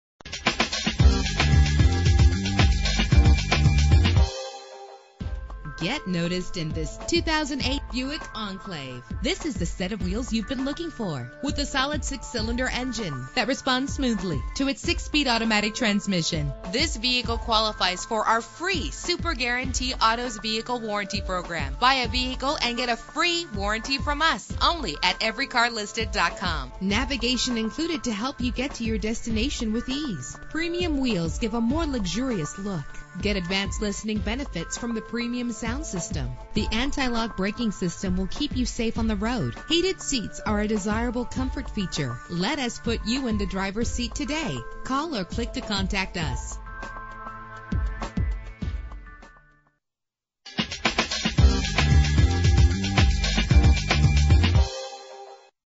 2008 Buick Enclave - Ephrata Pa
Year: 2008
Make: Buick
Model: Enclave
Trim: CXL
Engine: 3.6 liter 6 cylinder 24 valve
Transmission: 6-Speed Automatic
Color: Brown
Mileage: 49874
Address:
1928 West Main Street